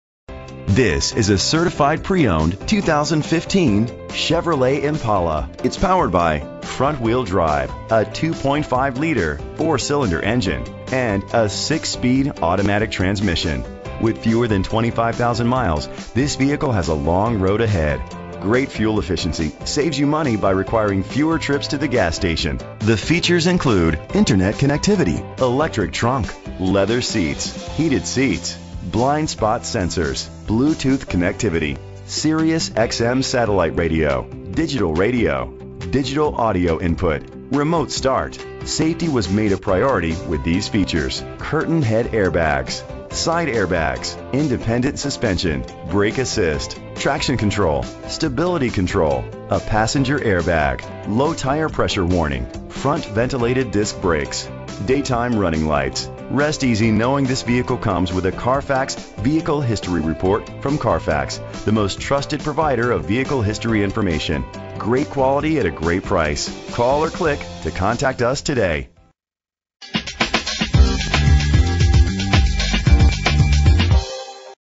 2015 Chevrolet Impala - Tinley Park IL
Apple Chevrolet

Year: 2015
Make: Chevrolet
Model: Impala
Trim: LTZ w/1LZ
Engine: 2.5 liter inline 4 cylinder 16 valve
Transmission: 6-Speed Automatic
Color: Silver Ice Metallic
Mileage: 21450

Address:
8585 W. 159th St.